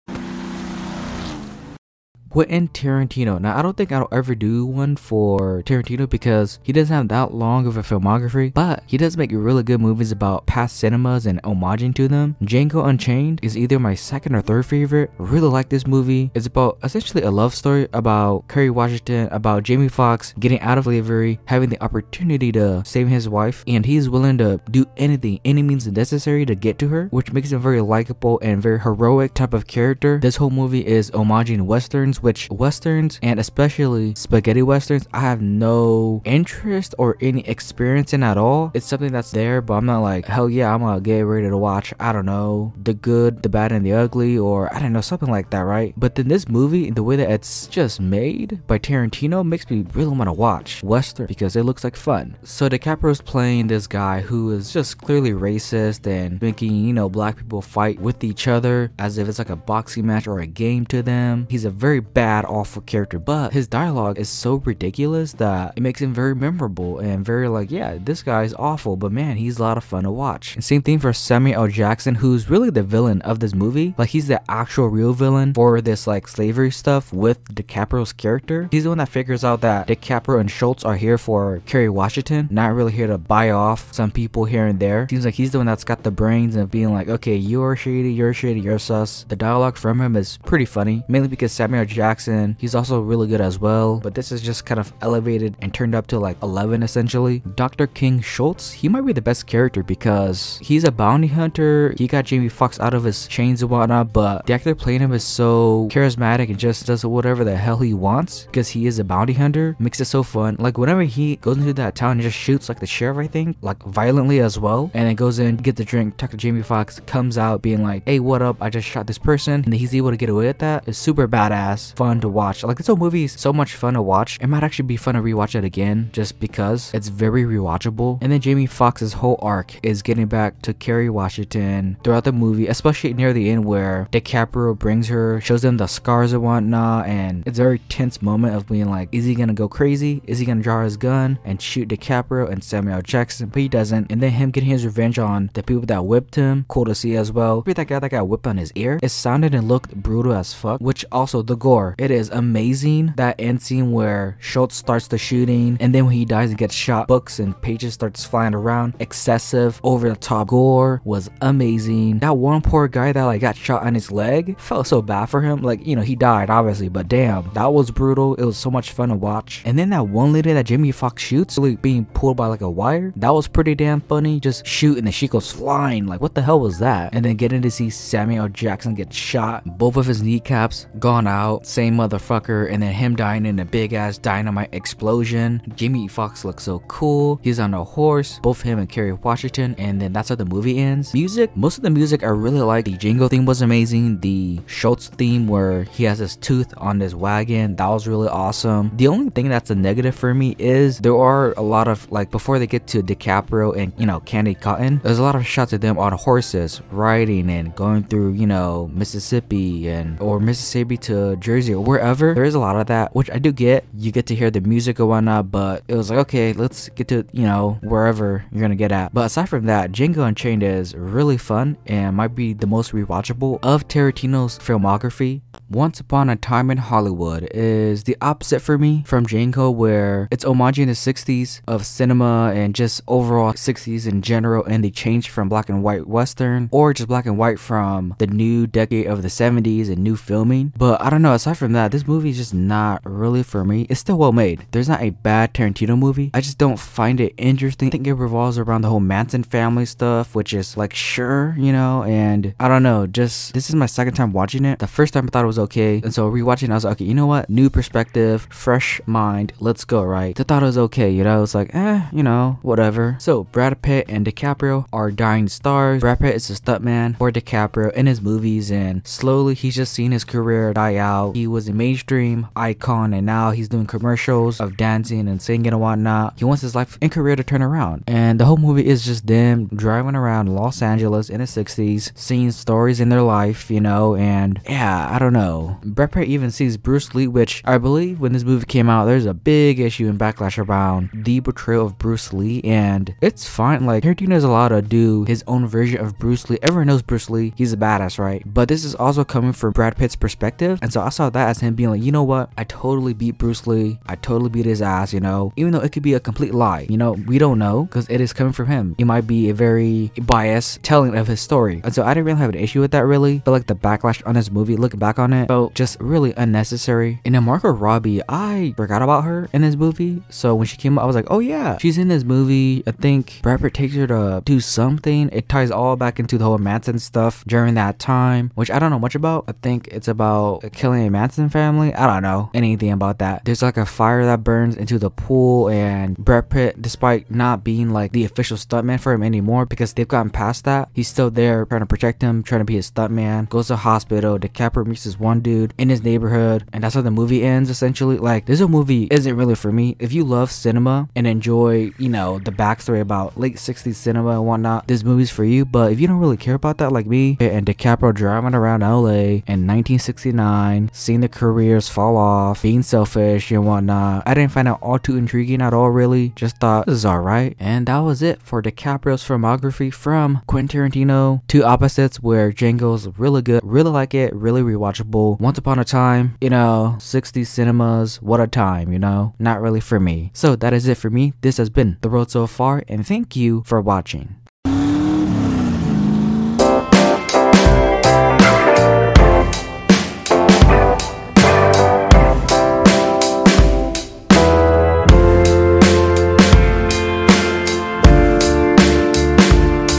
Leonardo DiCaprio's Filmography | Quentin Tarantino
0:00 - Django Unchained
3:46 - Once Upon a Time in Hollywood

This is the start of a new series. I'm going through Leonardo Dicaprio's filmography to lead up to Killers of the Flower Moon which as of writing this doesn't have a date yet but when it does I'll rank every film from DiCaprio's filmography. I'm also doing Scorsese and Nicolas Cage's filmographies along side with this as well. I'm doing this to broaden my taste in films.